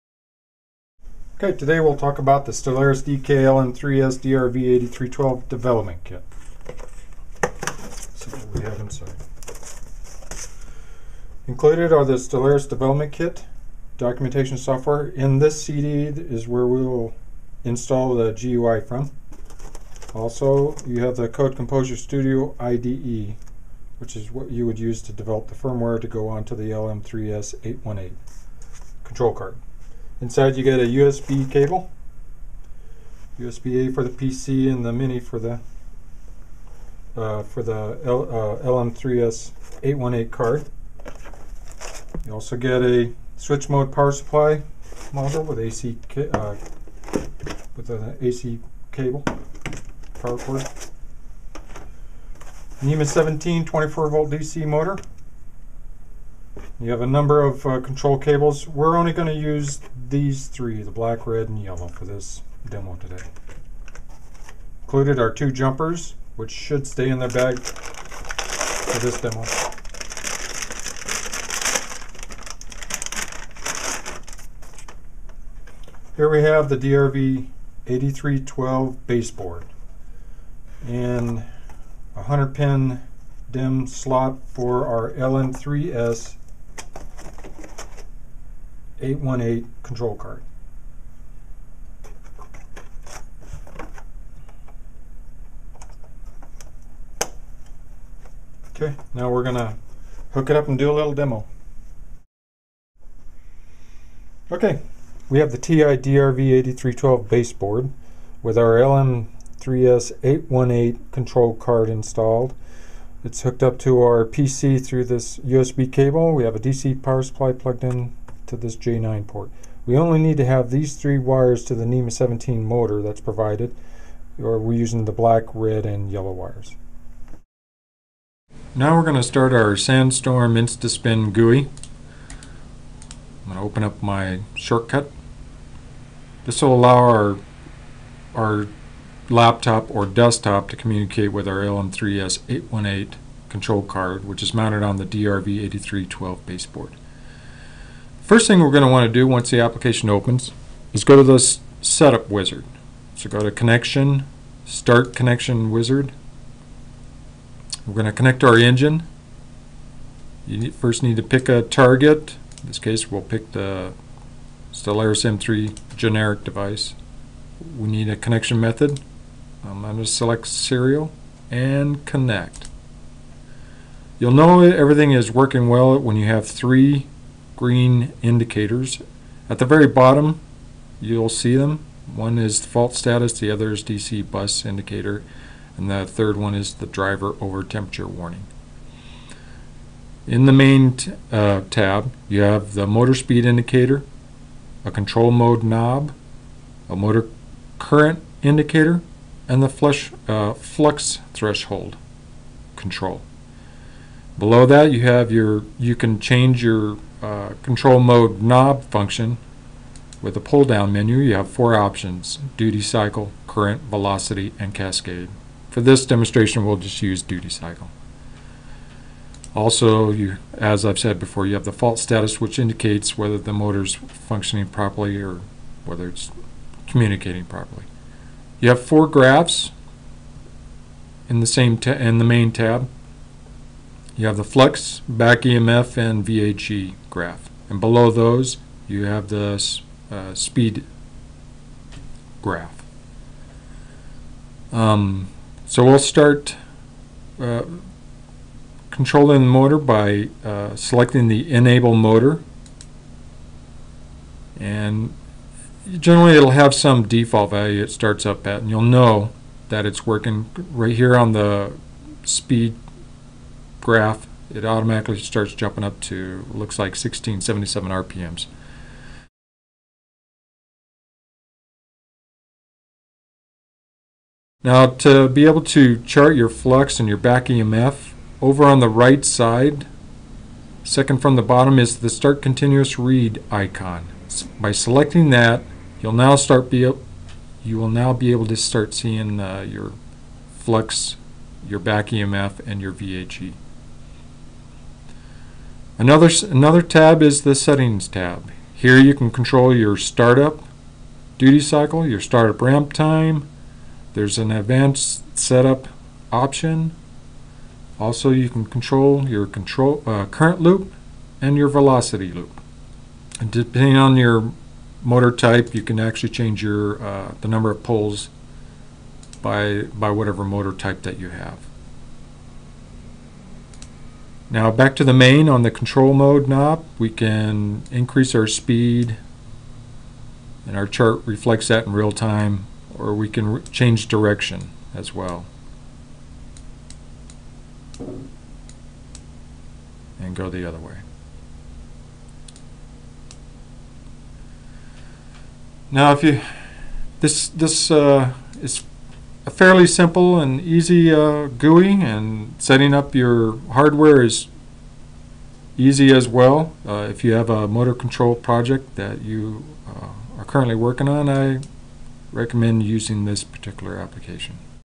Stellaris Development Kit from Texas Instruments - Product Overview
Bob Casiano reviews the Stellaris DK-LM3S-DRV8312 Development Kit from Texas Instruments.